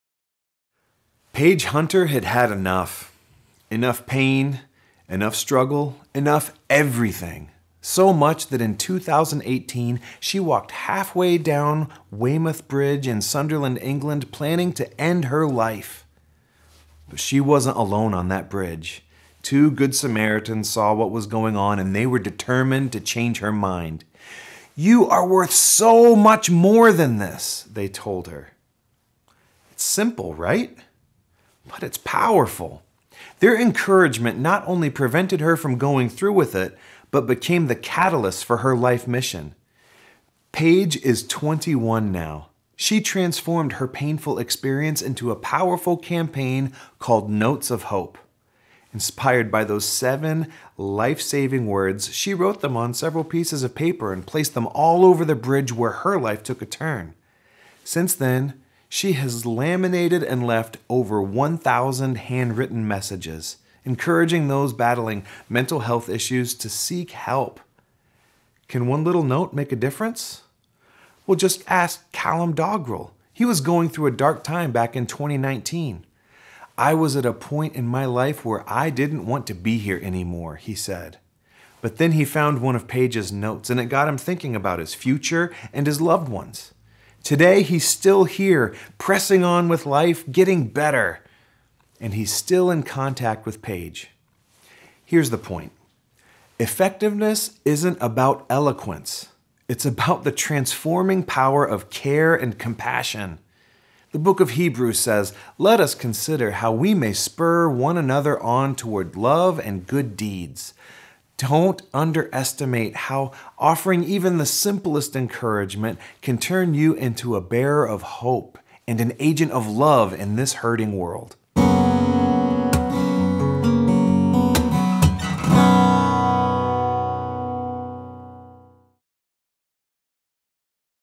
Minute with Mike (2/1/24)
Pastor Mike talks about the power of encouragement as he tells the story of a young woman whose lowest moment became her life's turning point.